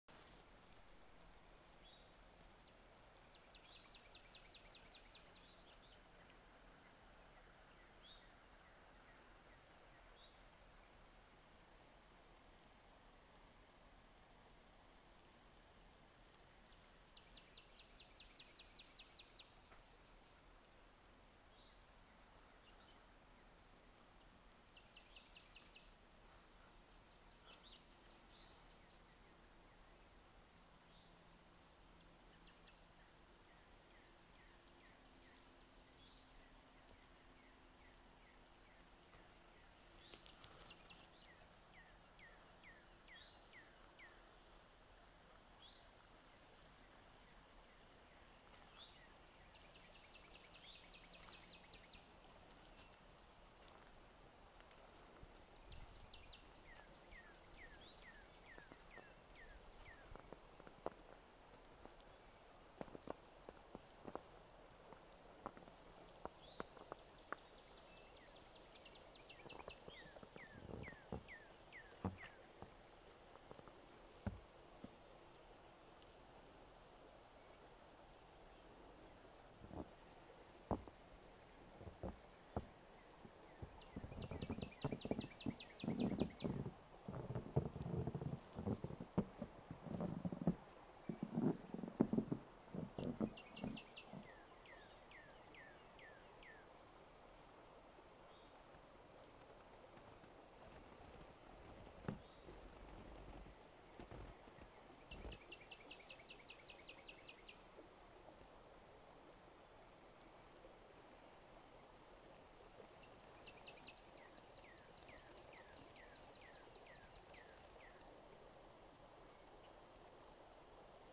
April Snow
A surprise April Snow made for a magical walk along the Butler-Freeport Community Trail near Marwood.